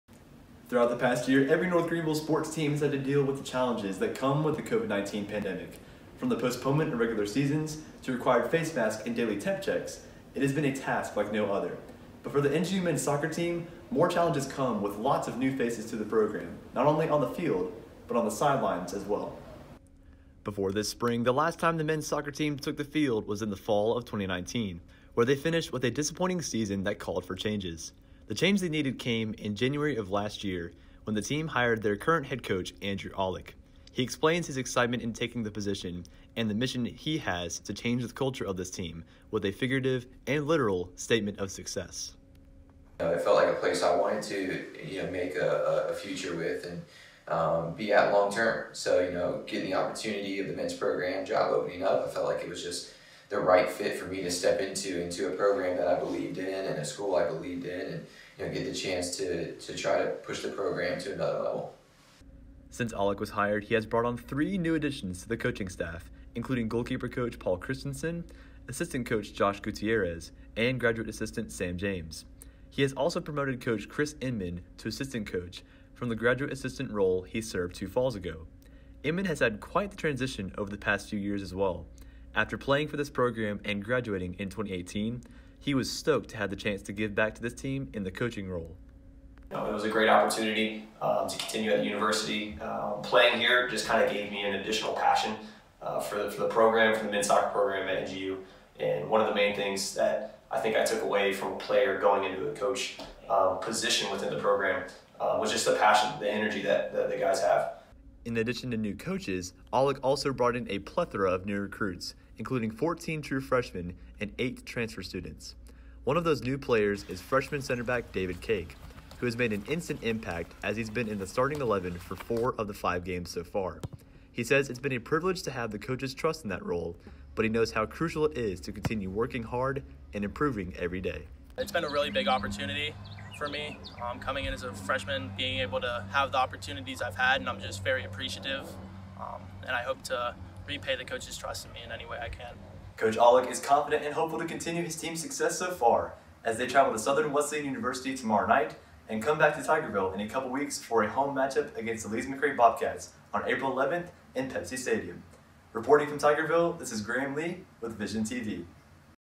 New Faces All Around - NGU Men's Soccer Team Takes the Field
Vision TV's Graham Lee shares the specific challenges North Greenville University Men's Soccer has faced after a long time away from the field and a swath of new faces as new coaches and players take the field.